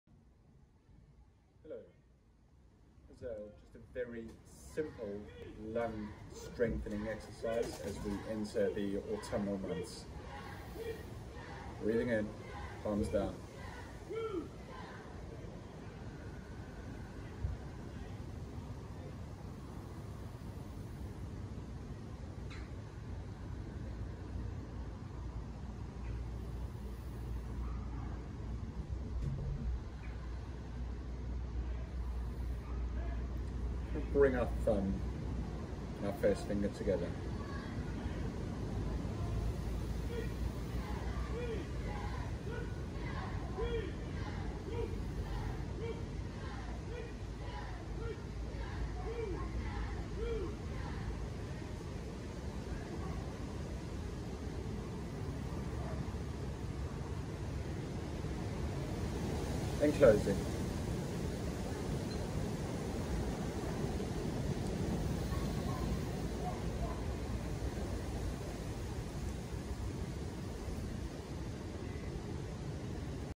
Simple lung strengthening exercise
A simple Qigong lung strengthening exercise as we move into autumn where lung strength and our immunity benefit from regular cultivation